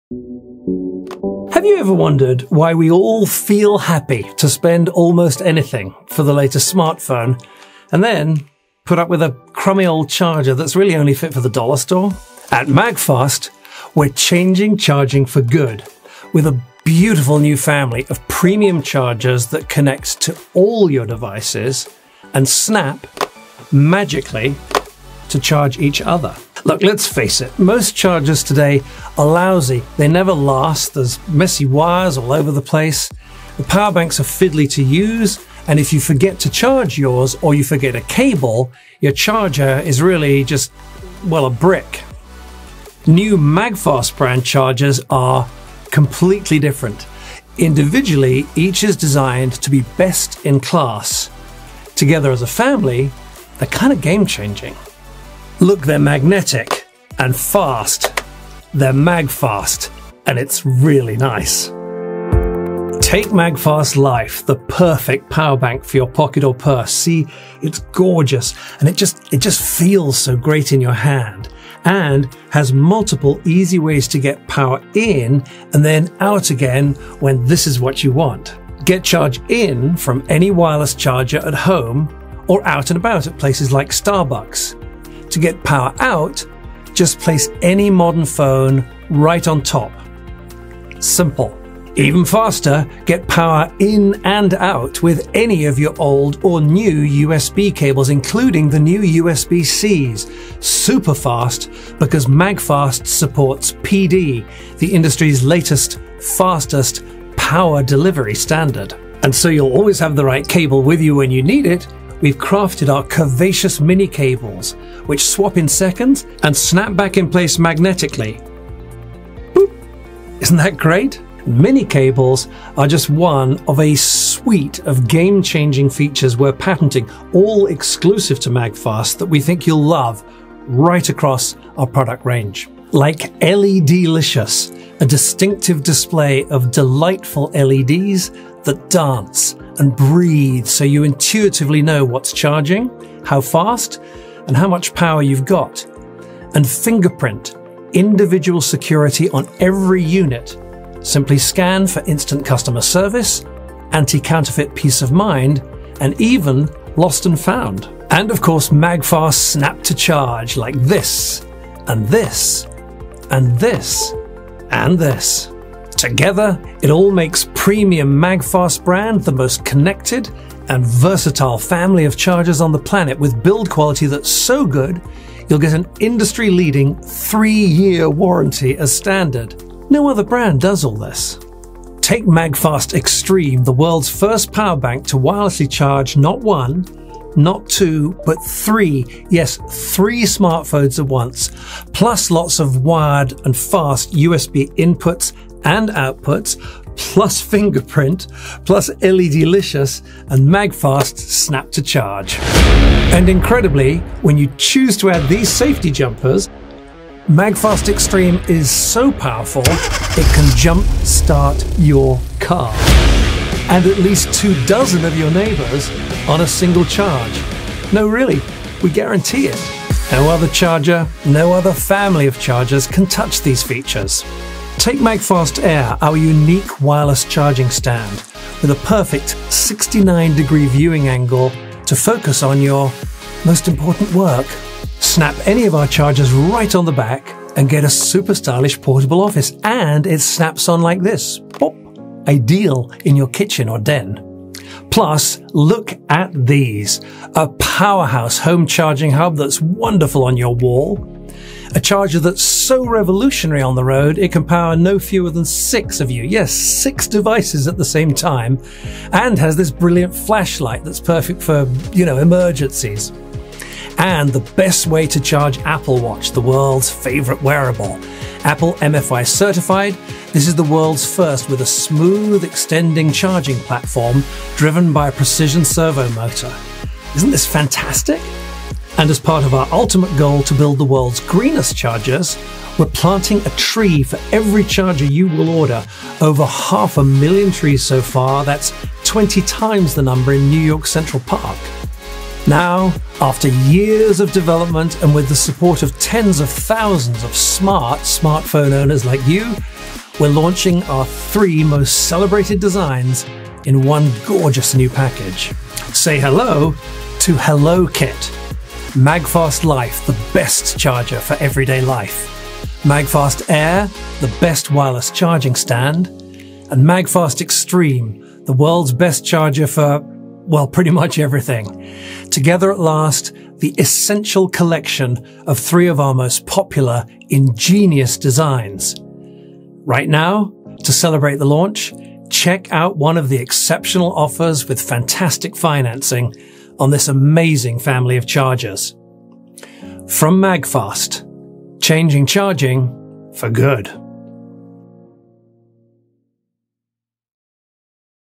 MAGFAST. Changing charging. For good.
MAGFAST Extreme is truly a new class of power bank, utterly uncompromising in every way.

Extreme's sleek, patent-pending design encloses a powerhouse of charging tech. The first power bank to charge three wireless 'Qi' devices simultaneously it also has 5 full USB outlets and sports the latest 'PD Power Delivery' standard. Oh yes. Add the optional Jumpers, and Extreme can jump-start your car... plus up to two dozen of your neighbors... on a single charge.